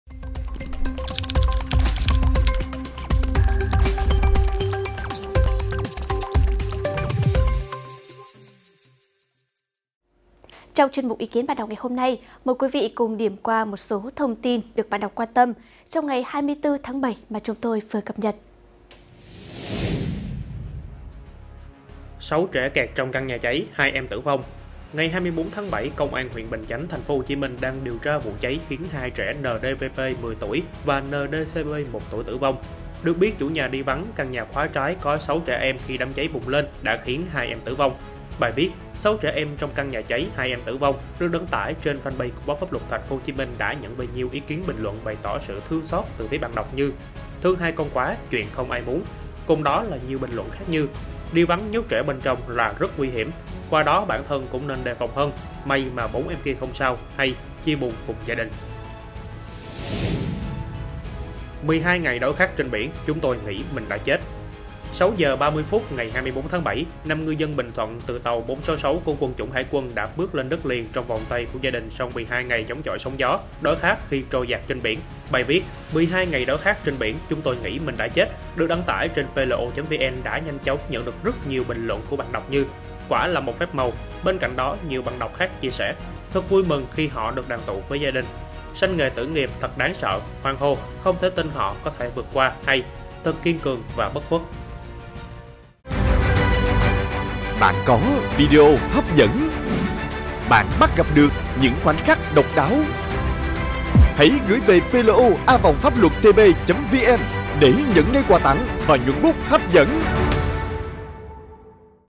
Ý kiến bạn đọc: 6 trẻ kẹt trong căn nhà cháy, 2 em tử vong - PLO
Ý kiến bạn đọc 25-7: 6 trẻ kẹt trong căn nhà cháy, 2 em tử vong; '12 ngày đói khát trên biển, chúng tôi nghĩ mình đã chết'...